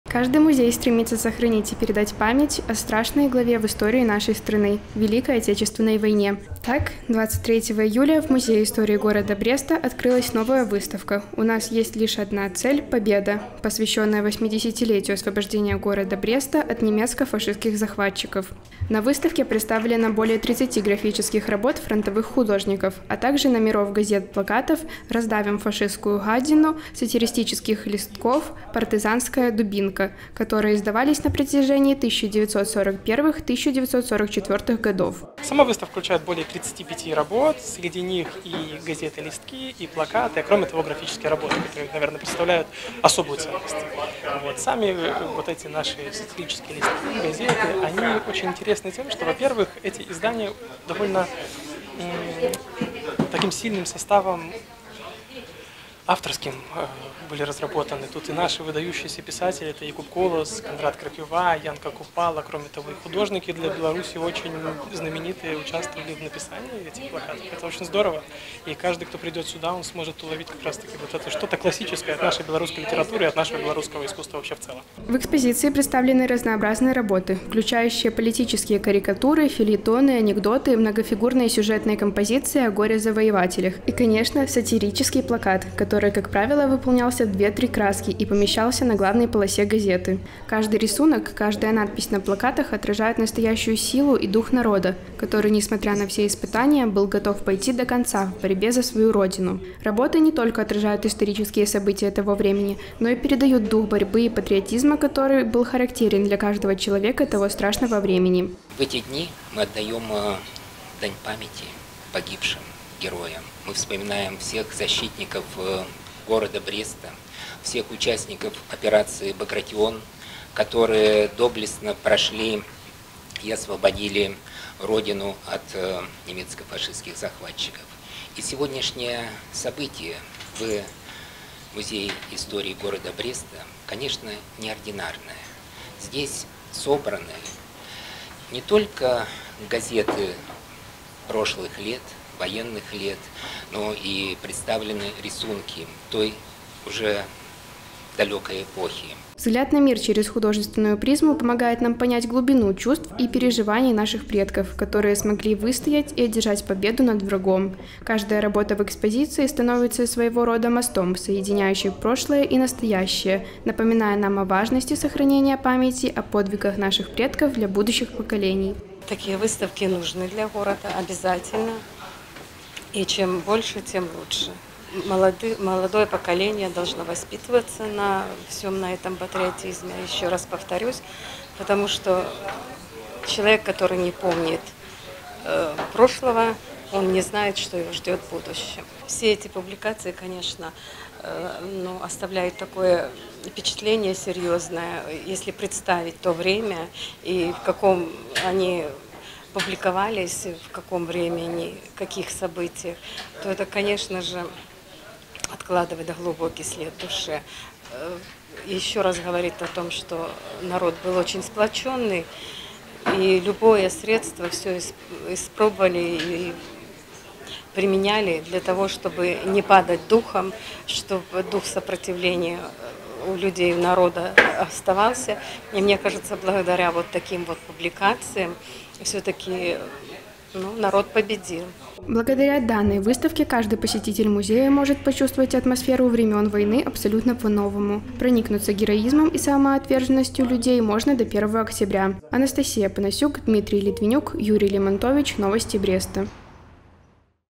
2024-07-24 Выставка «У нас есть лишь одна цель – Победа».
«У нас есть лишь одна цель – Победа» – эти слова стали не только названием выставки, но и лозунгом, напоминающим о непоколебимости духа нашего народа. Выставка, посвященная 80-летию освобождения города Бреста от немецко-фашистских захватчиков, предусматривает более 30-ти графических работ фронтовых художников. Посетить музей «Истории города Бреста» и окунуться в прошлое белорусского народа можно до 1 октября.

*Наши контакты:*
✅Телефон горячей линии:
54-63-67